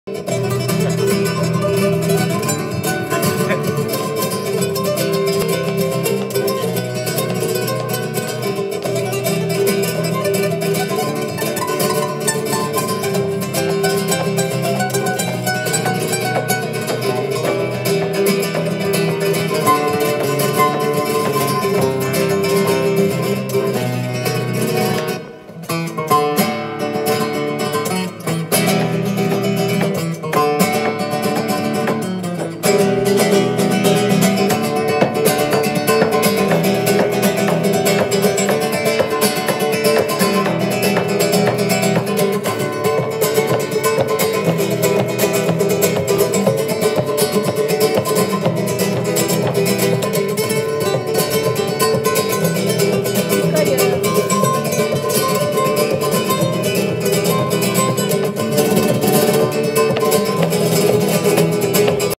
Flamenco en Vuelta
Eric Vaarzon Morel en beeldende kunst van Gerrit van Emous. Beauforthuis 2020